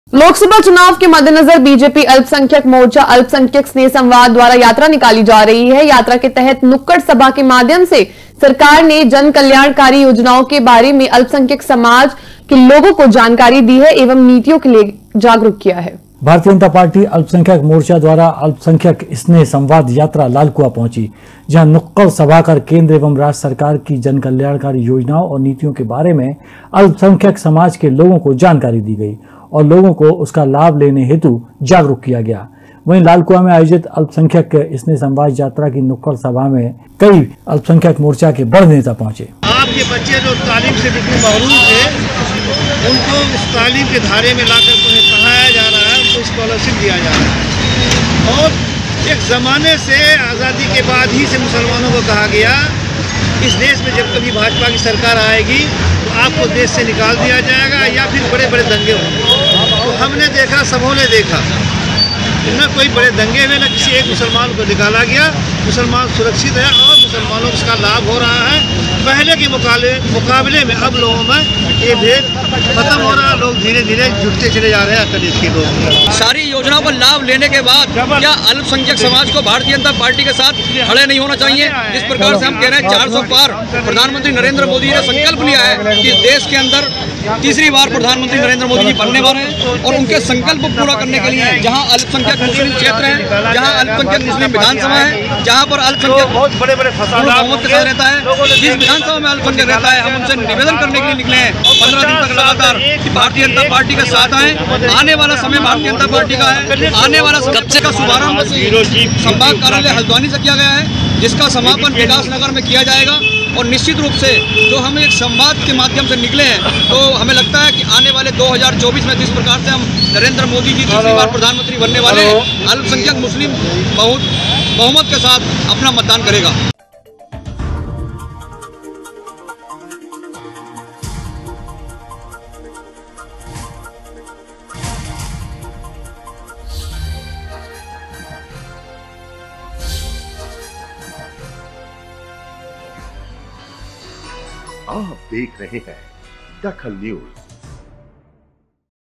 DAKHAL NEWS अल्पसंख्यक स्नेह संवाद यात्रा का शुभारंभ
अल्पसंख्यक स्नेह संवाद यात्रा का शुभारंभ
अल्पसंख्यक समाज को किया जा रहा है जागरूक